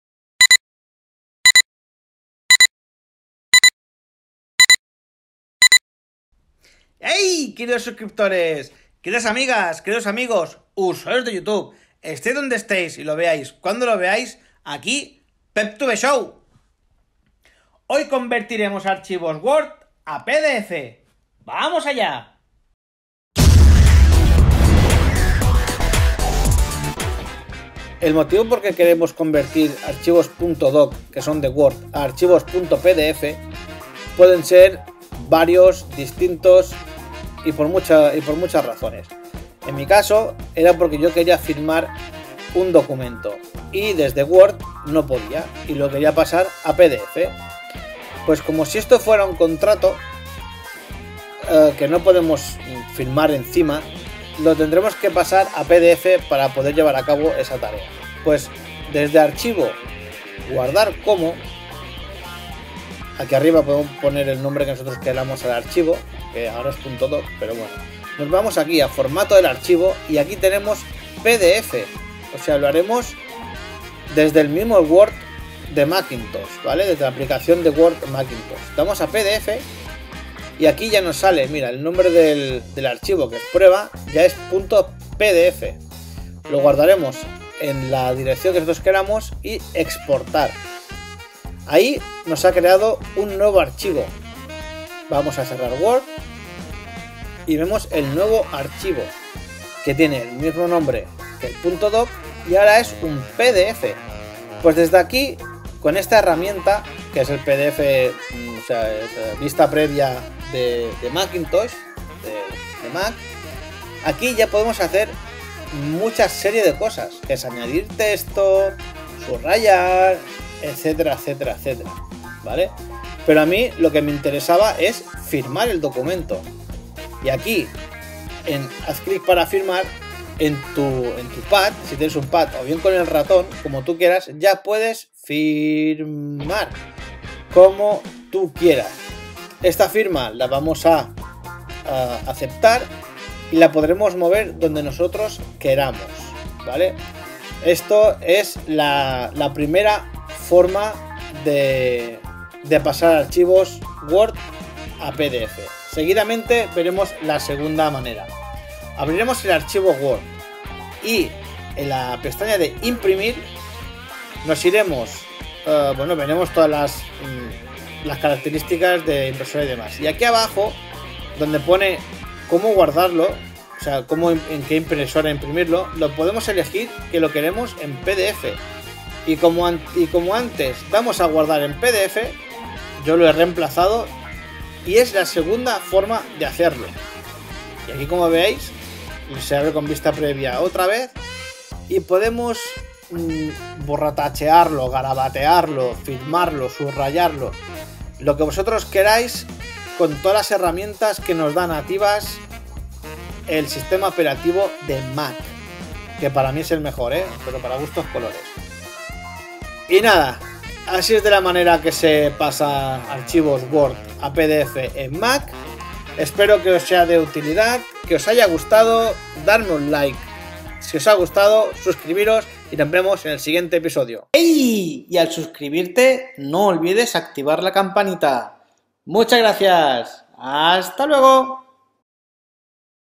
Convertir archivos WORD a PDF GRATIS en MAC (Word To PDF)
Vídeotutorial de cómo convertir archivos word a PDF en cualquier MacOS